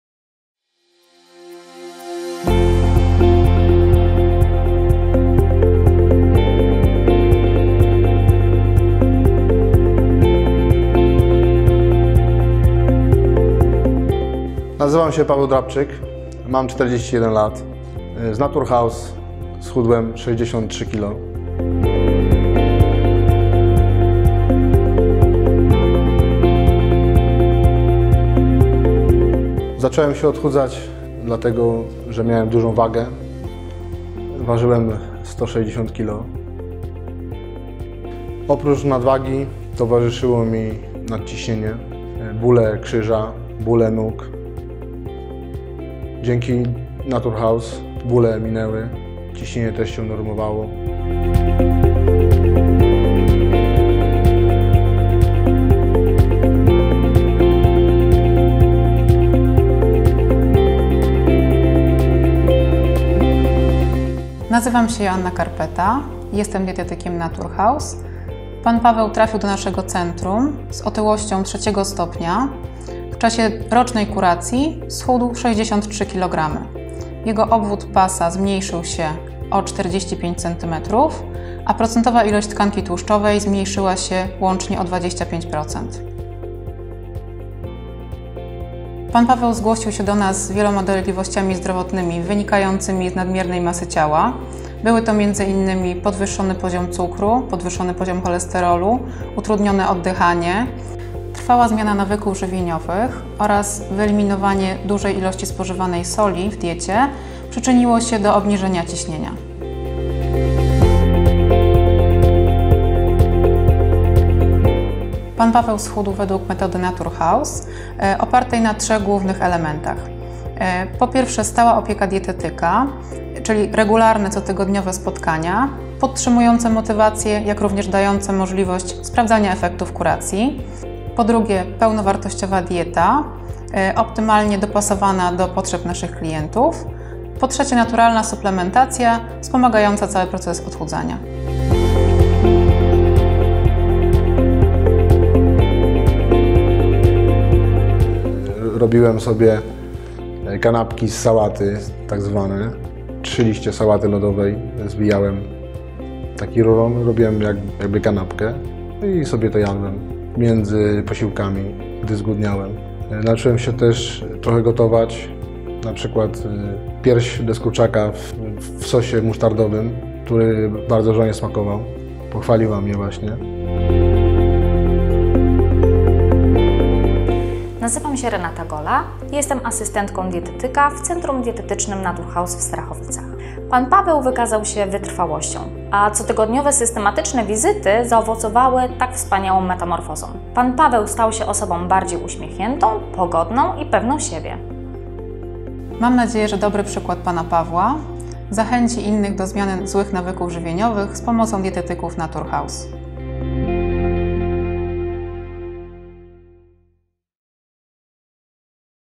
JAK STRACIĆ 63KG w 12 MIESIĘCY - SKUTECZNE ODCHUDZANIE
Pan Paweł ze Starachowic z powodu otyłości borykał się z wieloma problemami zdrowotnymi, takimi jak bóle kręgosłupa, trudności z oddychaniem, podwyższony poziom cukru we krwi, zbyt wysoki cholesterol, problemy z poruszaniem się. Dzięki odpowiednio dobranej kuracji odchudzającej w Naturhouse schudł 63 kg. Problemy zdrowotne ustąpiły i Pan Paweł znów cieszy się aktywnym życiem.